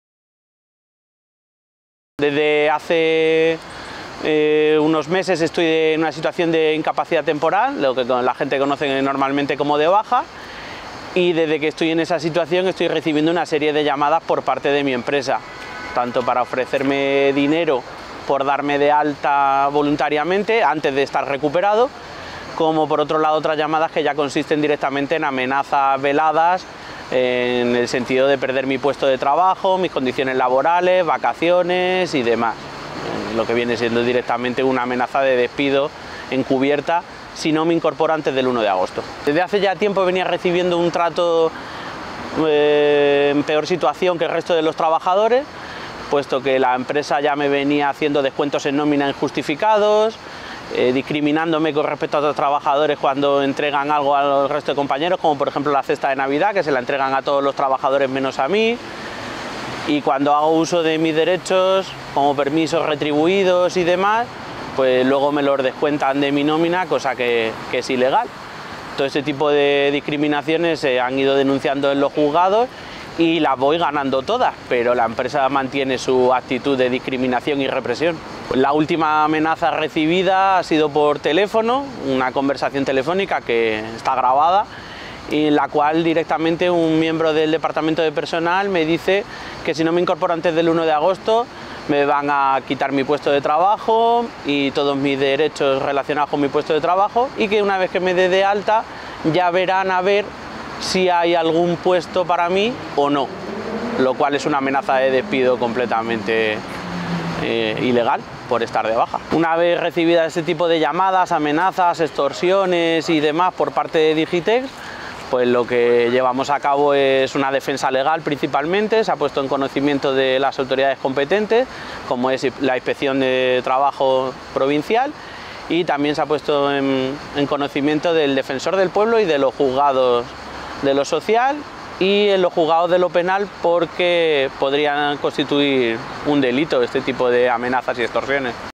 José Gabriel de CGT, en lucha con Digitex
José Gabriel, un delegado de CGT hostigado por Digitex, empresa donde trabaja.
Amenazas, discriminaciones, chantajes, etc. Es lo que sufre este trabajador, estando de baja médica.
Nos lo cuenta él mismo en este vídeo.